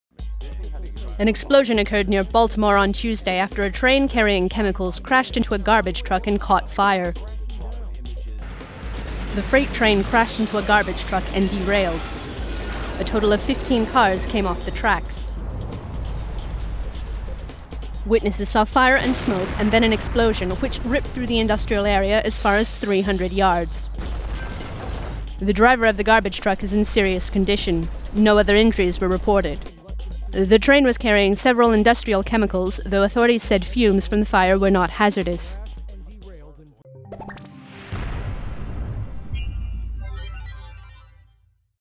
Train derails and explodes after crashing into garbage truck near Baltimore
A freight train carrying chemicals crashed into a garbage truck, then derailed and caught fire. Fifteen cars came off the tracks. Moments later, an explosion reaching 300 yards  occurred.